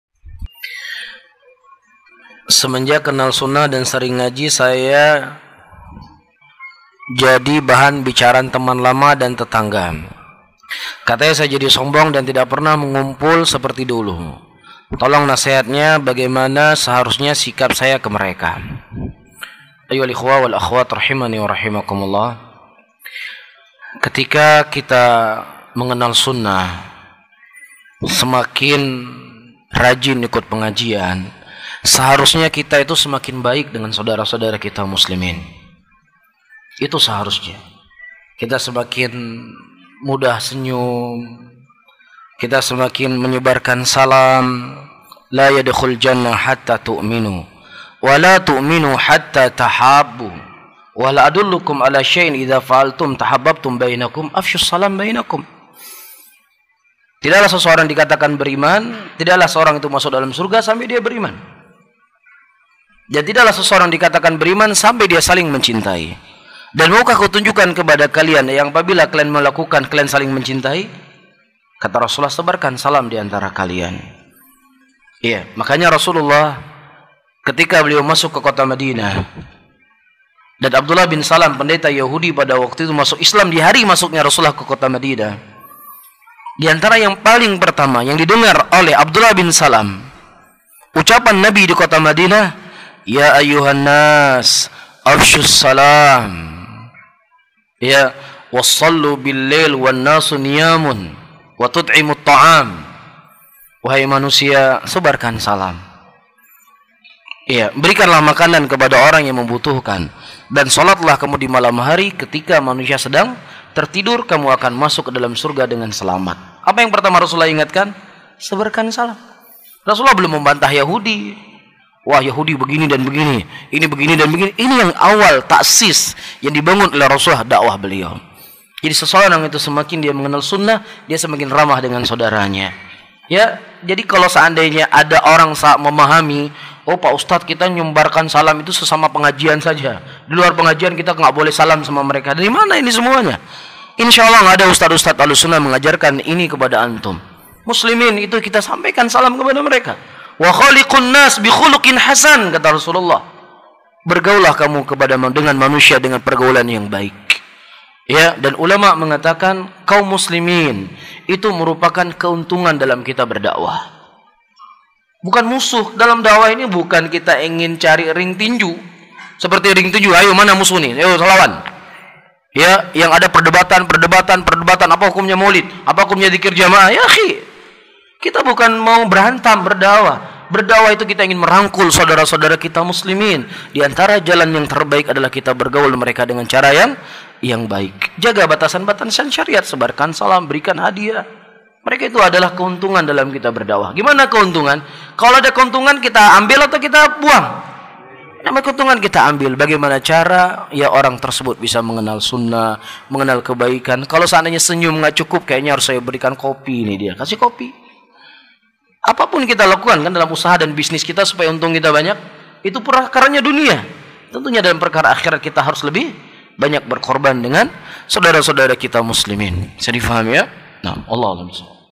Semenjak sering mengaji dibilang sombong oleh teman & tetangga 🔵 Ust. Harits Abu Naufal
Bismillah..

Semoga bermanfaat.